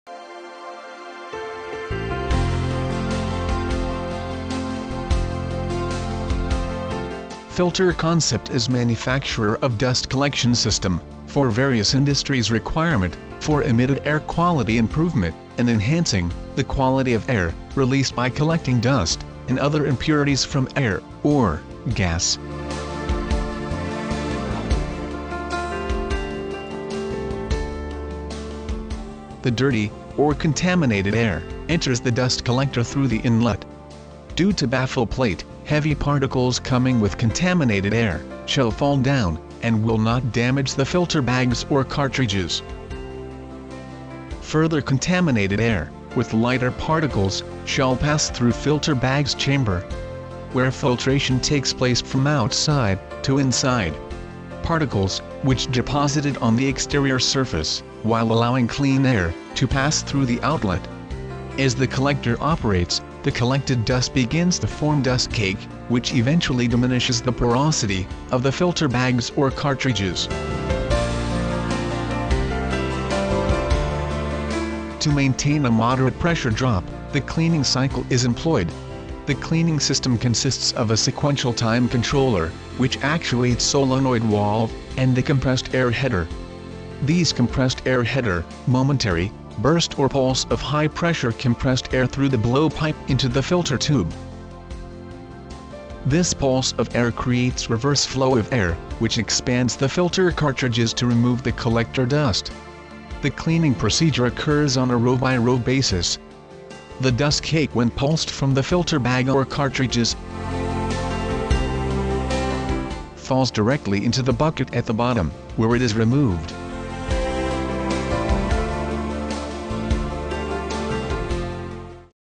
Dust Collection Systems | Pulse Jet Dust Collection Systems - Manufacturer India
Filter Concept is manufacturer of Pulse Jet Dust Collection system for various Industries requirement for air quality improvement and enhancing the quality of air released by collecting dust and other impurities from air or gas by removing particulate matter from the air and environment. We are a manufacturer of highly customized units to accurately match your Application requirements with customized ducting solutions along with our dust collectors for your specific dust collection requirements.